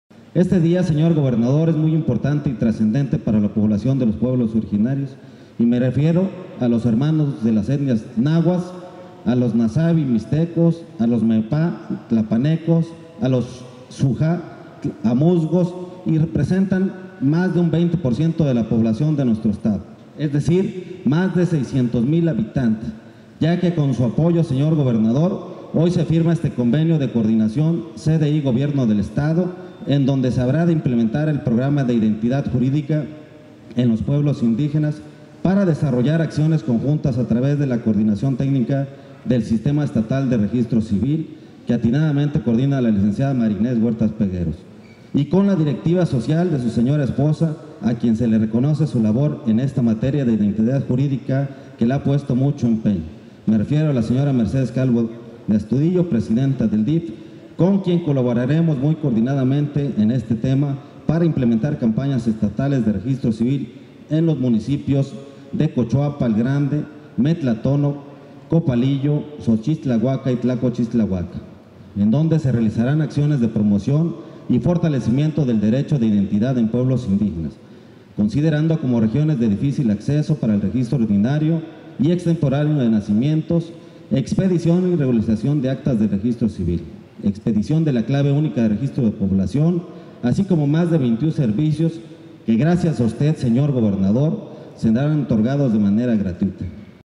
GradoCeroPress EL DELEGADO DE CDI EN GUERRERO,LENIN CARBAJAL CABRERA 120617
CONTINUAN EL GOBIERNO ESTATAL Y FEDERAL REALIZANDO MÁS ACCIONES A FAVOR DE LA POBLACION INDIGENA DE GUERRERO

• Firma Astudillo Flores y Carbajal Cabrera, convenio de Colaboración con CDI en materia de identidad jurídica.

Junio del 2017

Continuando con la ruta de trabajo y coordinado con el gobierno federal para beneficio de la población guerrerense a través de acciones y programas, el gobernador de Guerrero, Héctor Astudillo Flores firmo el Convenio de Colaboración entre el Gobierno de Guerrero y la CDI en materia de identidad jurídica.

“Es sin duda el gobierno de Guerrero un tocador de puertas permanente. El estado no puede detenerse en ningún momento, tenemos que seguir haciendo cosas para que este estado camine y camine de la mejor manera”, señalo el mandatario guerrerense que fue acompañado durante la entrega de apoyos del delegado de CDI, Lenin Carbajal Cabrera.

Y continuó su intervención, enfatizó que él gobierno que él encabeza, trabaja todos los días y seguirá trabajando junto con el gobierno federal y la sociedad civil, con el mismo ánimo y entusiasmo para enfrentar no solo los problemas que se viven cotidianamente trabajando arduamente en un estado que tiene las posibilidades de seguir adelante, “La creatividad que tienen los guerrerenses sin duda es muy notable hay que seguirla fomentando”; dijo Astudillo Flores.

Detalló que con el programa de Identidad en los pueblos indígenas, con una aportación estatal de 4 millones 793 mil pesos se desarrollarán acciones conjuntas entre CDI y la Coordinación Técnica del Sistema Estatal de Registro Civil para instrumentar una campaña estatal en los municipios de Cochoapa el Grande, Metlatónoc, Copalillo, Xochistlahuaca y Tlacoachistlahuaca, para el fortalecimiento de la identidad en pueblos indígenas, para el registro ordinario, extemporáneo de nacimientos, CURP, pero también habrá una campaña permanente en Tlapa, Ometepec, entre otros municipios.

Astudillo Flores, hizo entrega de recursos para poner en marcha proyectos productivos a Indígenas, que contará con una inversión de 30 MDP este año, con la finalidad de aumentar la productividad y la integración al mercado de productos elaborados por indígenas guerrerenses, fomentando el autoempleo y mejorar su producción, productividad, e inclusión social.

Astudillo Flores, anuncio la puesta en marcha en breve de la campaña de Derecho a la Identidad que consiste en que gente de la zonas indígenas, principalmente de las regiones más apartadas de Montaña puedan obtener beneficios en trámites de Registro Civil de manera gratuita, previéndose que durante este 2017 serán entregadas 19 mil actas de nacimiento en las zonas más apartadas del estado; Reconociendo el gobernador Héctor Astudillo la noble labor de Registro civil y del DIF Guerrero que encabeza Mercedes Calvo de Astudillo

Asimismo, se destacó que el gobierno estatal que encabeza Astudillo Flores ha realizado 157 mil servicios en todo el estado entre aclaraciones, rectificaciones, registro de nacimiento, matrimonio, así como de Curp “yo tengo mucha confianza que este programa, va hacer unos de los más importantes que del gobierno de Héctor Astudillo”, recalco el mandatario guerrerense en su mensaje.
(lunes 12 de junio 2017)